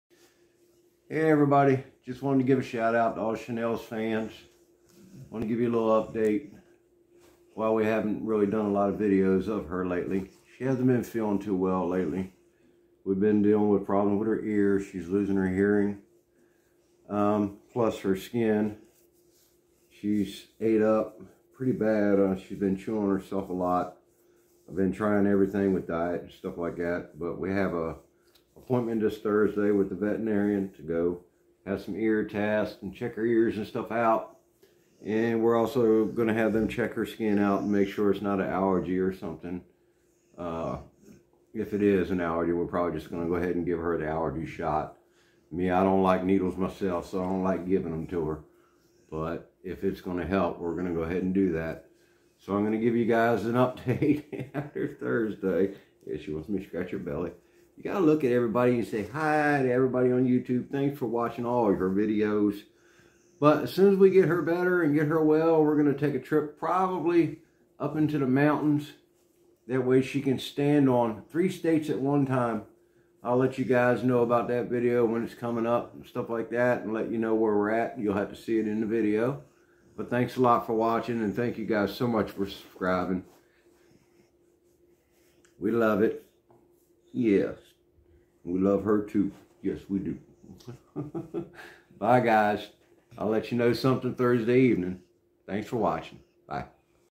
pittie Chanel update , why we haven't done a video of her lately.
update on Chanel's condition and why we have been doing videos of her lately.